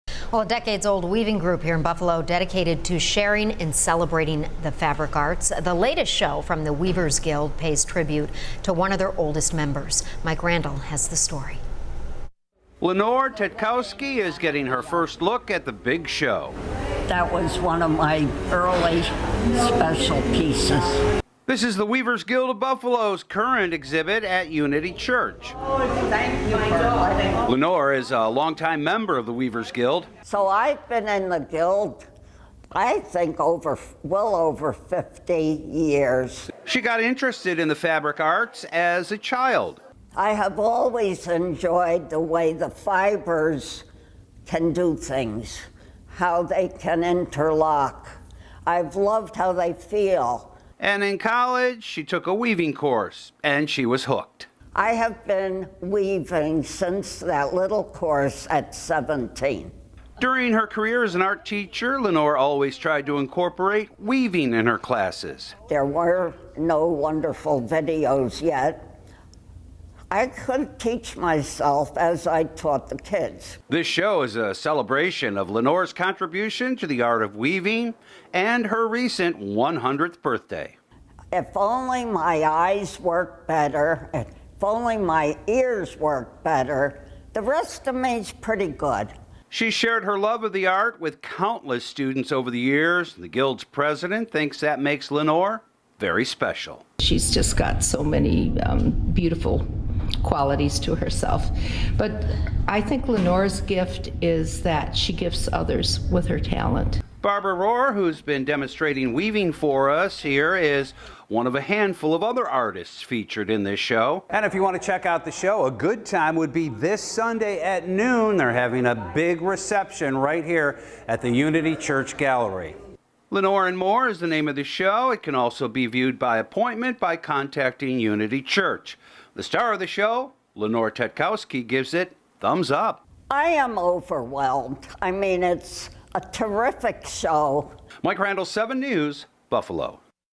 The Weavers Guild of Buffalo celebrates Lenore Tetkowski's talent and 100th birthday
Lenore Tetkowski is being celebrated for her talent and her 100th birthday at the Weavers Guild Of Buffalo's current show at the Unity Church Gallery called "Lenore and More".